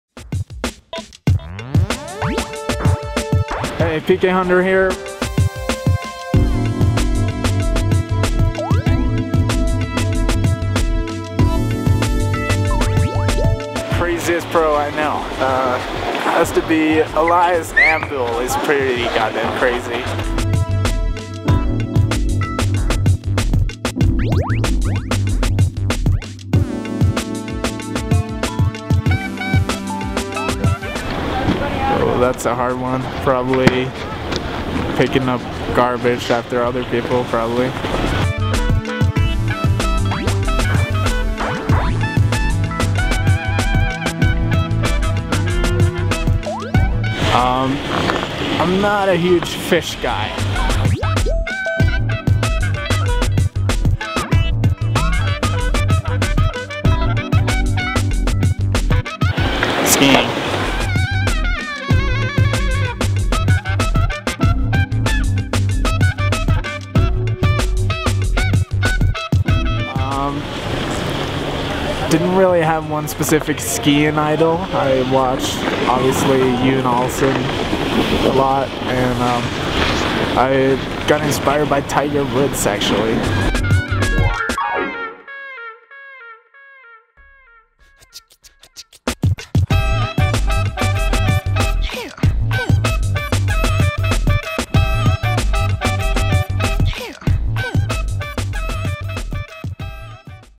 Freeskier PK Hunder - Five Questions & Five Tricks
What would PK Hunder be doing if not professionally freeskiing? Picking up garbage. His first job was skiing, and hopefully it will be his last. In this Alli Sports My Five, Hunder mentions his experience in the work field and how Tiger Woods was his childhood idol. Hear Hunder's five while watching him run the slopestyle course at Breckenridge.